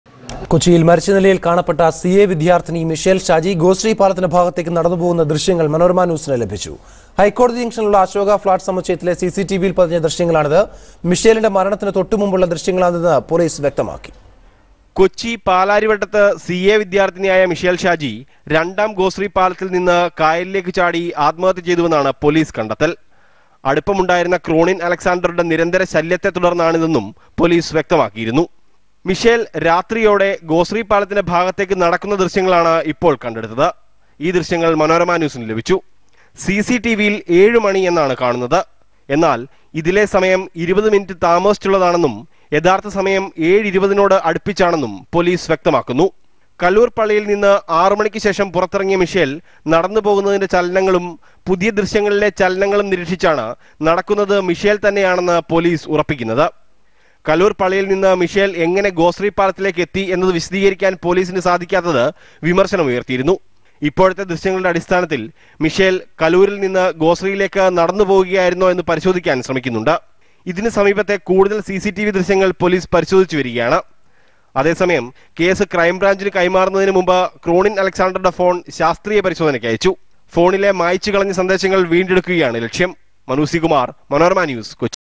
CCTV visuals of Michele Shaji walking to Gosree Bridge | Manorama News
CCTV footage of Mishel's final moment